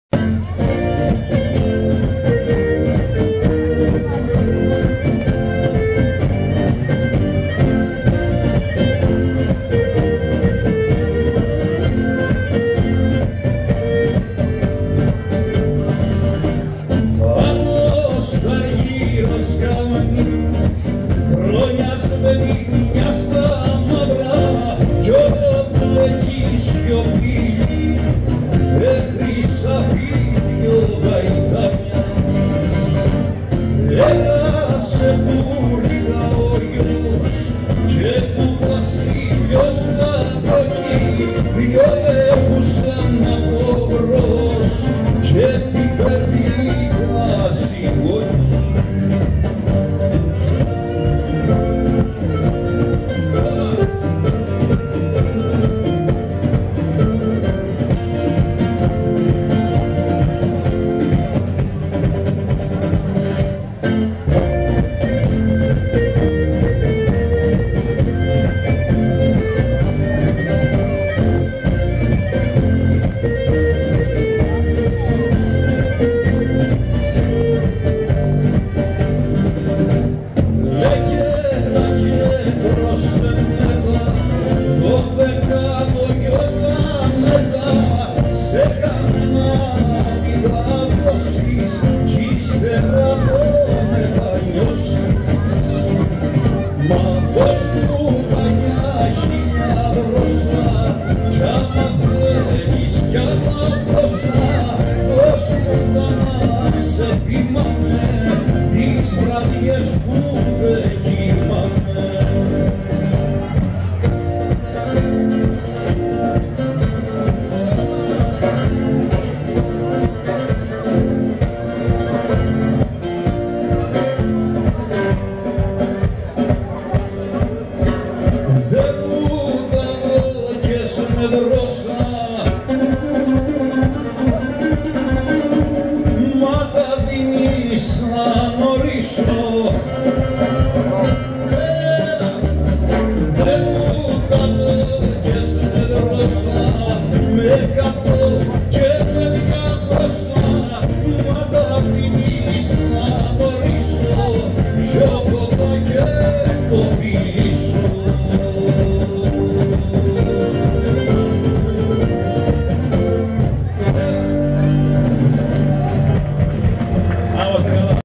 Γιώργος Νικηφ.Ζερβάκης Κρητικός Γάμος Φαιστός Παλάς
Ο Γιώργος Νικηφ.Ζερβάκης σε Κρητικό γάμο στο φαιστός Παλάς
4 7 2015.
Η λήψη έγινε με Nikon Coolpix L830 Σε αναλυση 1920Χ1080HD.